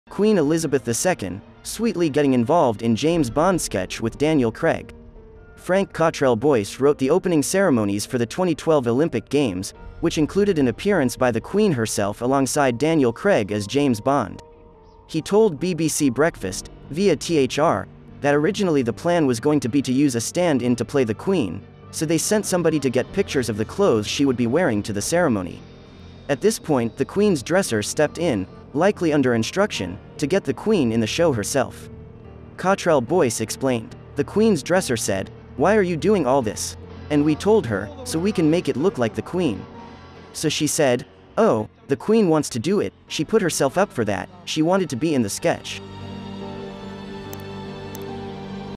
An interesting fact about Queen Elizabeth II | James Bond
Queen Elizabeth II, Sweetly Getting Involved In James Bond Sketch With Daniel Craig

Frank Cottrell-Boyce wrote the opening ceremonies for the 2012 Olympic Games, which included an appearance by the Queen herself alongside Daniel Craig as James Bond. He told  BBC Breakfast (via THR) tha originally the plan was going to be to use a stand-in to play the queen, so they sent somebody to get pictures of the clothes she would be wearing to the ceremony. At this point the Queen’s Dresser stepped in, likely under instruction, to get the Queen in the show herself. Cottrell-Boyce explained…

The Queen’s Dresser said, ‘Why are you doing all this?’ And we told her, ‘So we can make it look like the Queen.’ So she said: ‘Oh, the Queen wants to do it,’ She put herself up for that, she wanted to be in the sketch.